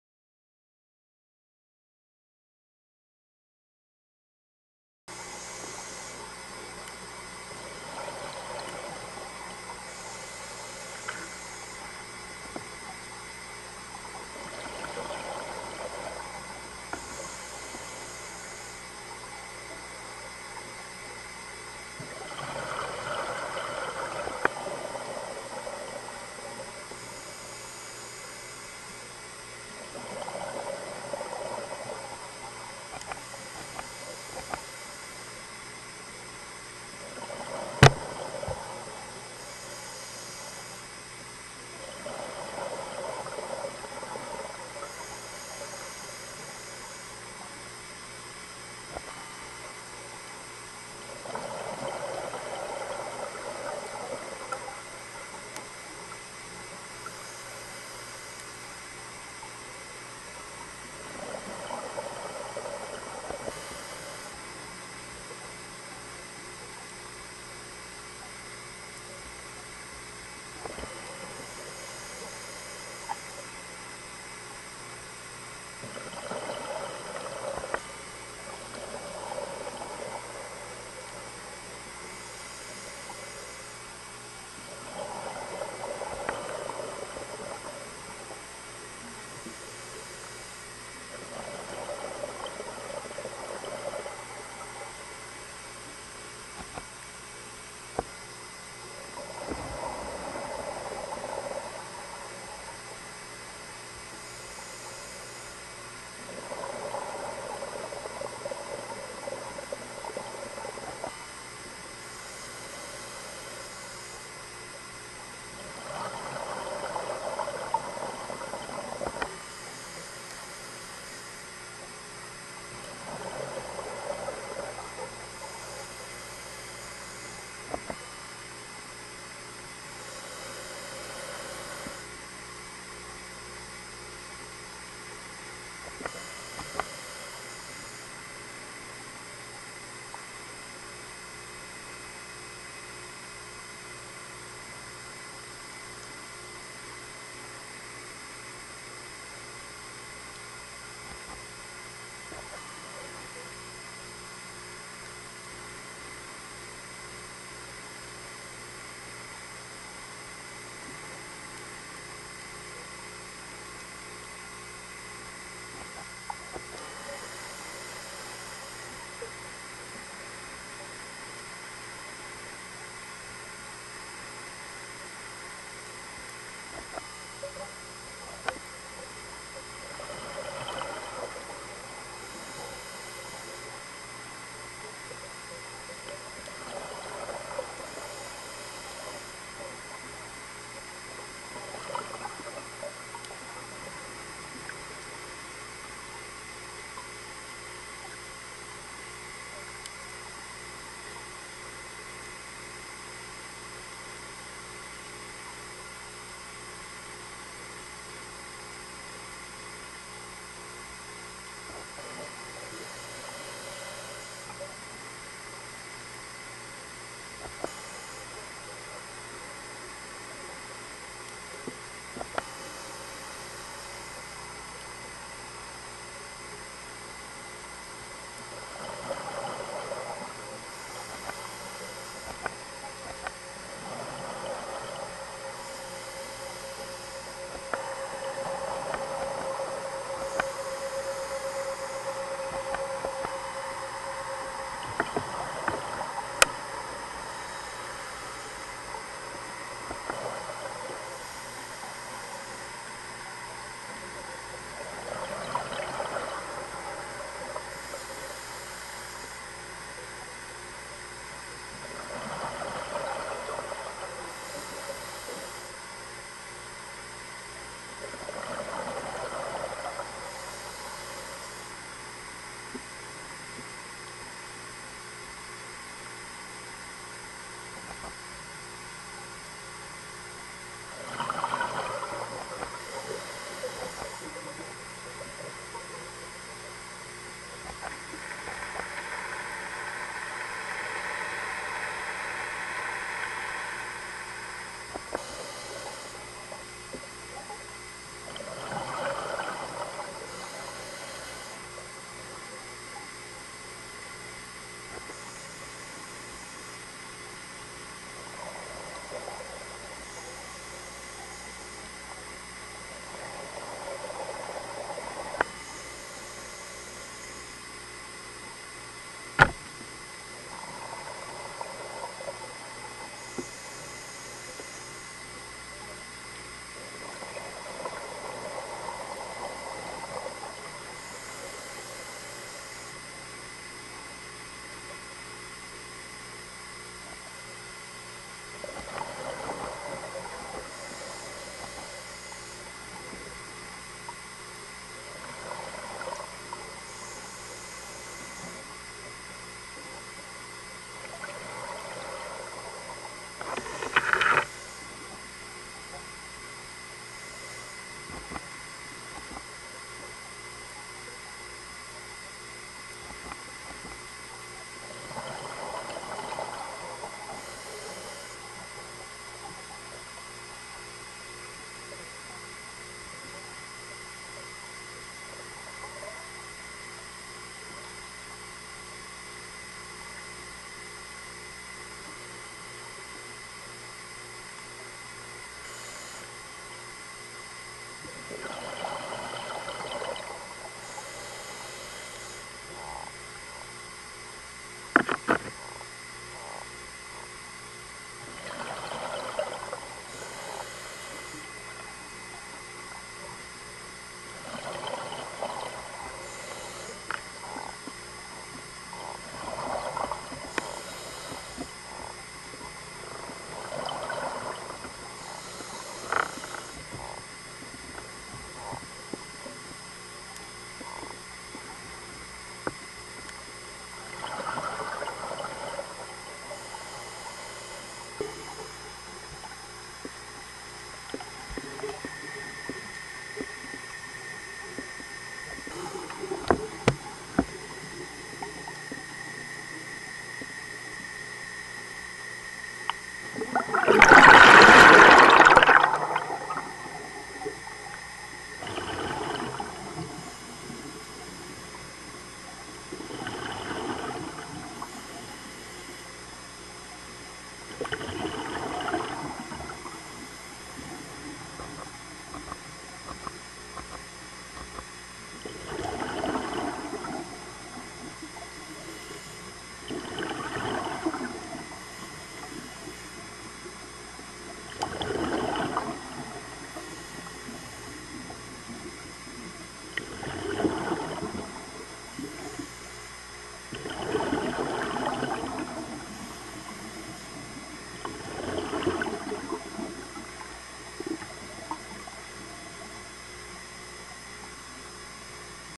7/23/2005 Red Grouper In The Sherwood Forest Coral Reef Footage, Dry Tortugas National Park.
7/23/2005 Red Grouper In The Sherwood Forest Coral Reef Footage, Dry Tortugas National Park. Video of a Red Grouper playing with the camera and swimming around the camera.